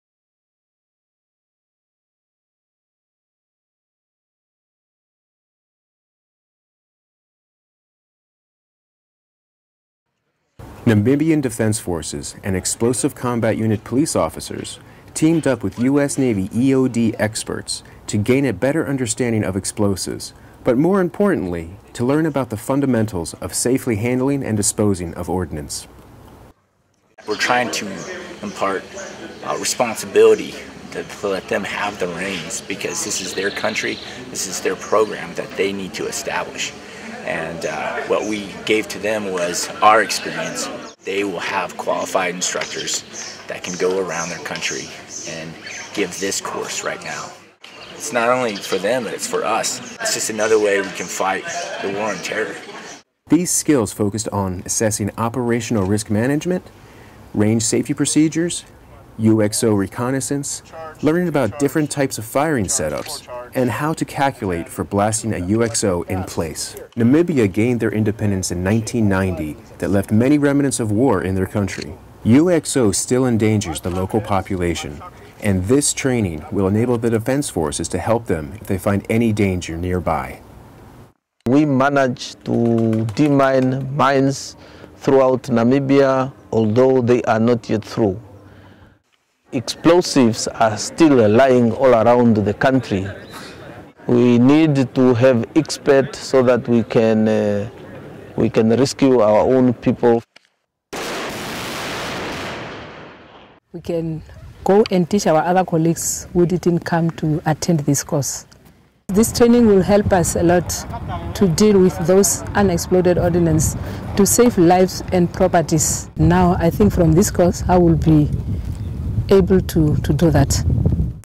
UXO Training given by U.S. Navy in Namibia.mov
U.S. Navy Explosive Ordnance Disposal (EOD) technicians, from Combined Joint Task Force - Horn of Africa, teamed with the Namibian Defense Force and Namibian Explosive Control Unit police officers recently for Explosive Ordnance Disposal (EOD) Level One and International Mine Action Standards (IMAS) training.